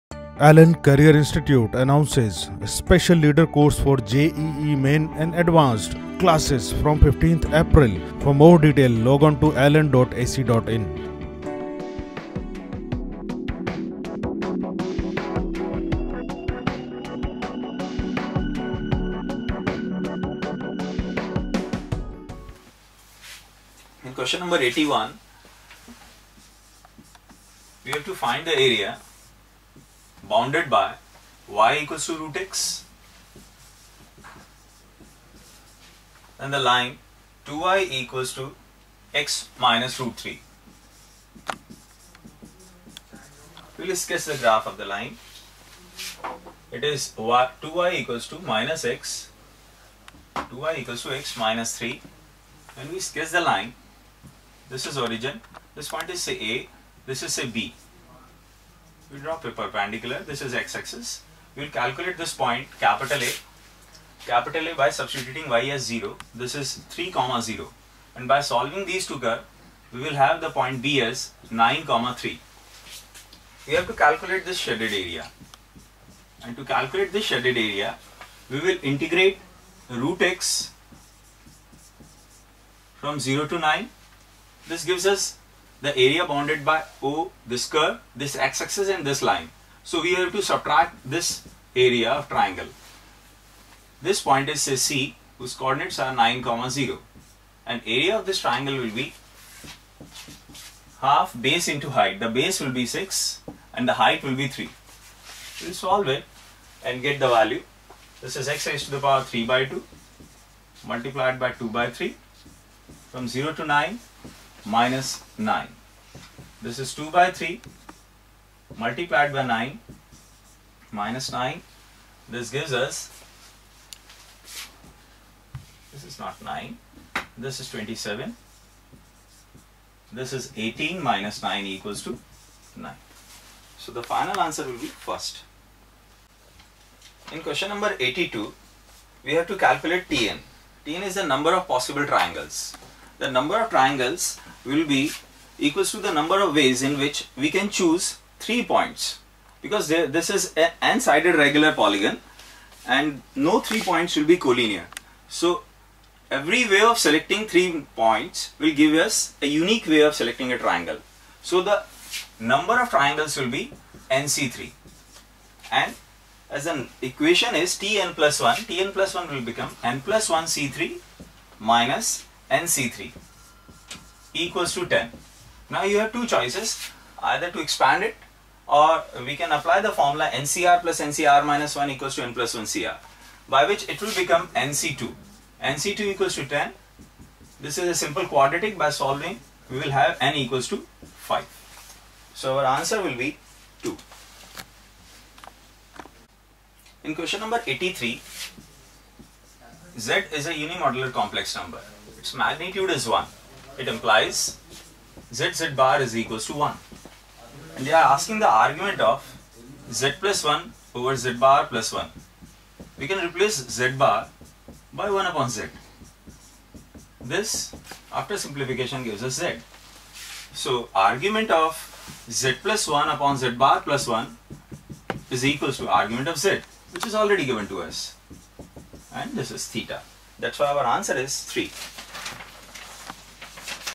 Mathematics Ques: 81,82,83
Video Solutions for JEE-Main 2013, by ALLEN Career Institute Kota, held on 7th April 2013.
Subject : Mathematics
Question nos. 81,82,83
Solutions done by Ashutosh Hisaria Sir (Faculty -ALLEN Career Institute)